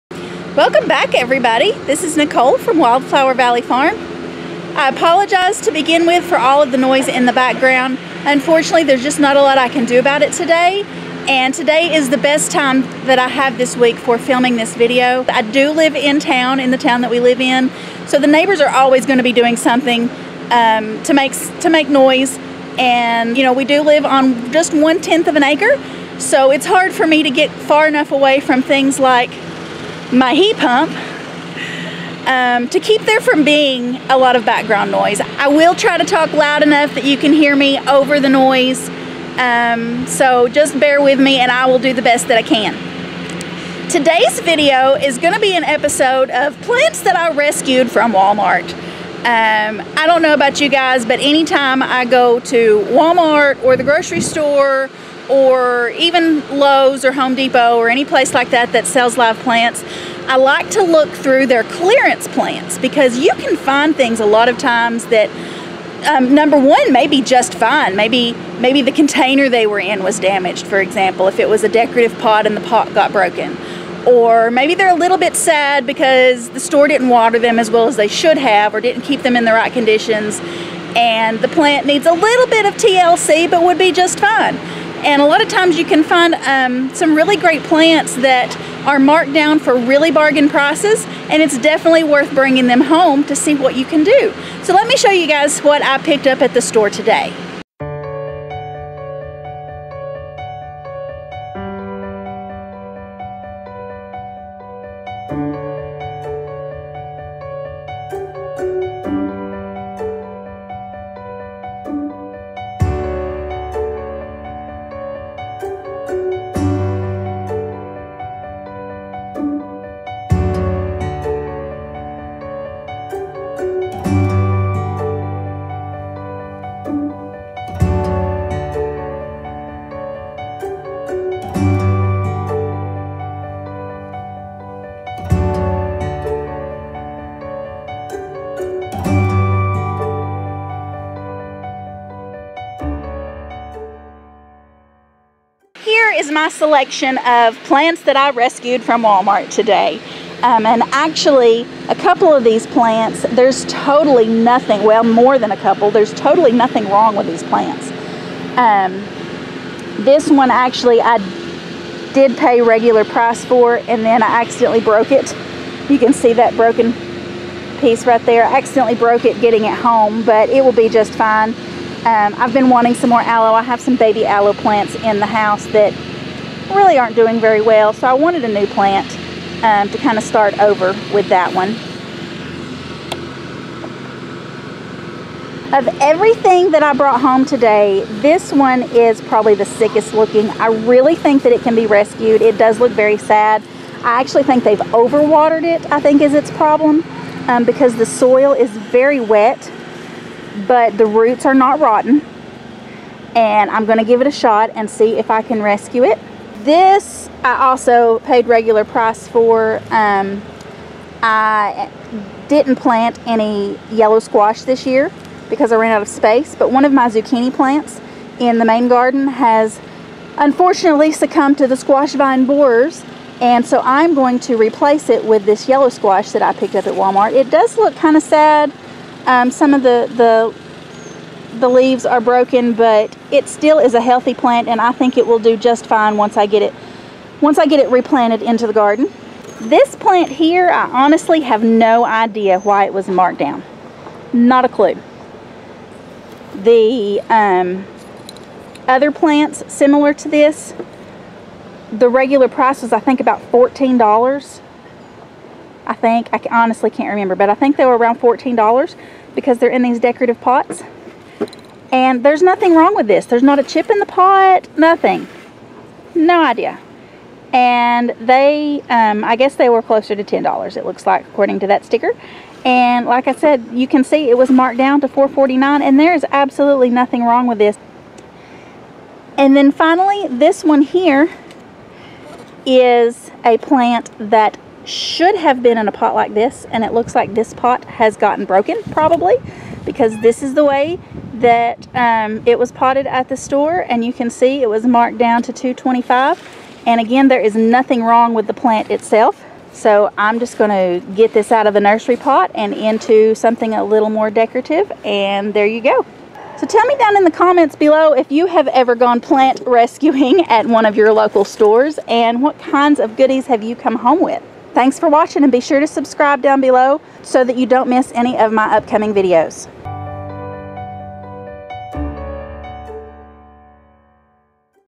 Plants I rescued from Walmart
I cannot walk past the discounted plants at any store without taking a look.